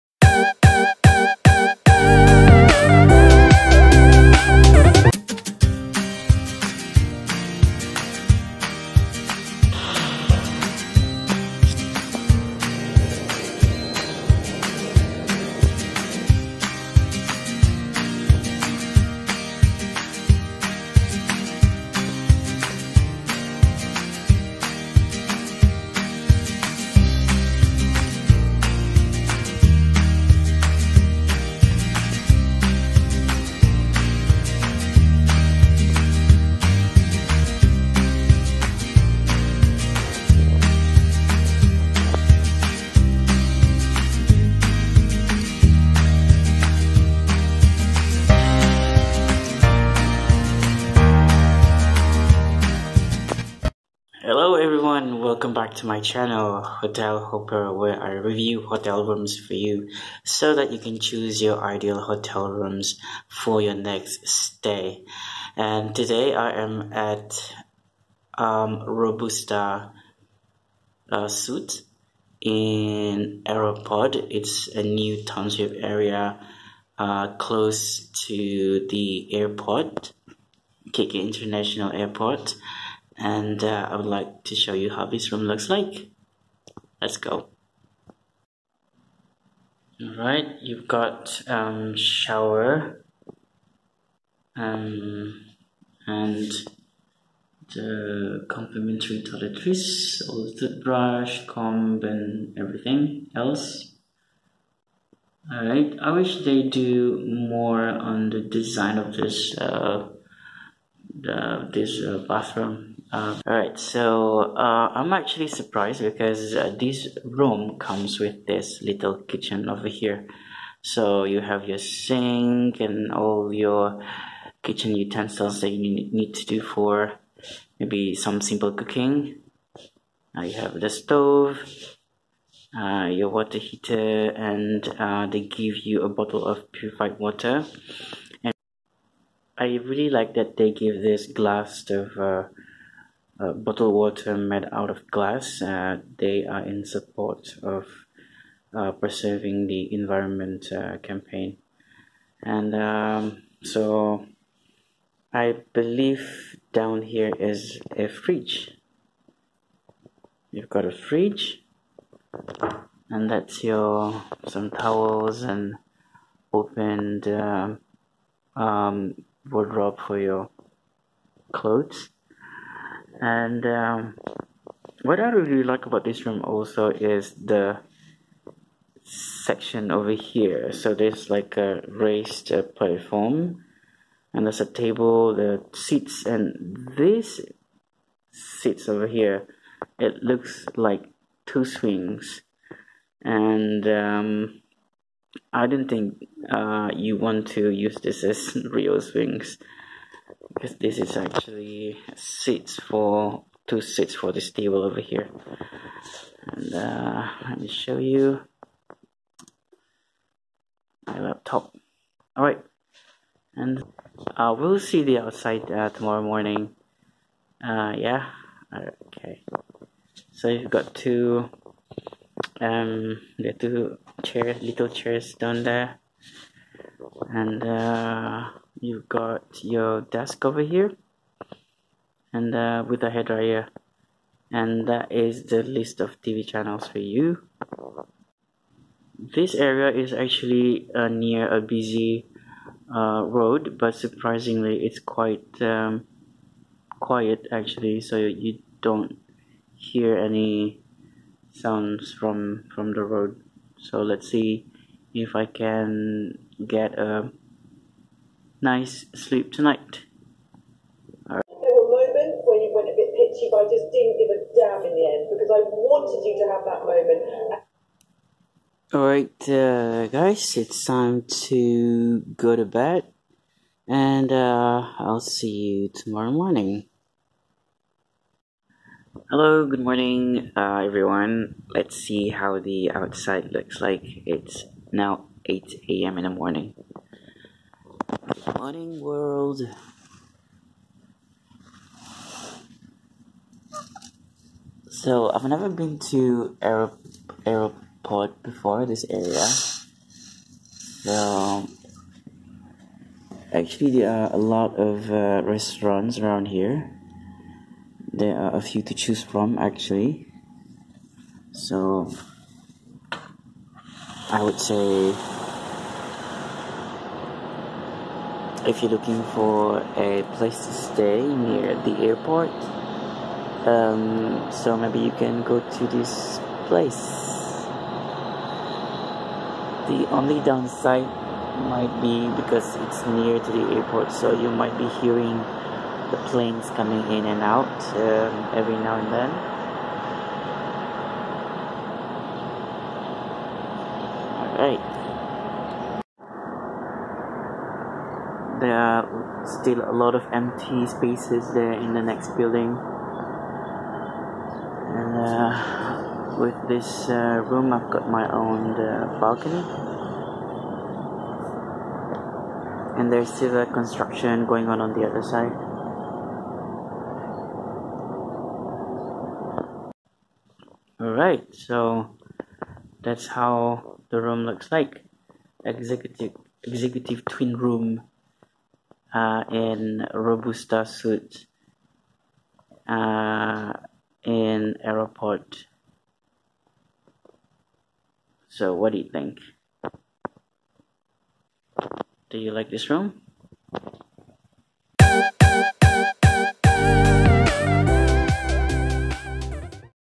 Need place to say near KKIA? RobustaSuite: Aeropod. Kota Kinabalu hotels review.
I got the room for MYR195* (online booking). Therea are cafes, restaurant and 24 hours convenient stores in the area.